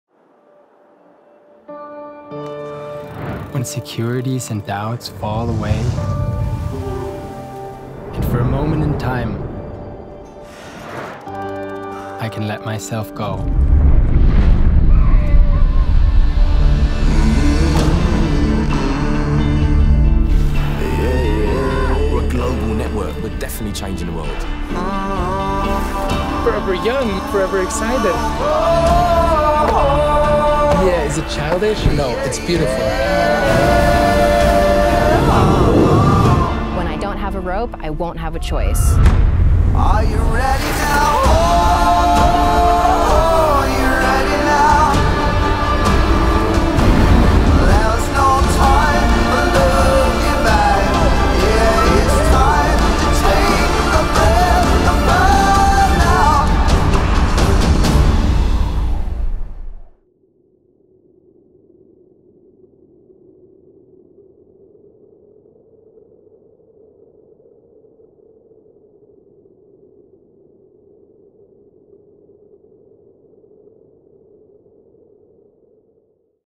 E.O.F.T. 19/20 – OFFICIAL TRAILER
WATCH IT NOW: With the Official Trailer we really kick into gear for the European Outdoor Film Tour 19/20! Our brand-new programme is packed with one-of-a-kind stories of pioneering adventurers.
No casting, no make-up, no actors – true to our motto THIS IS REAL!
Get a taste and watch 60 seconds from the best film studio we know: The great outdoors!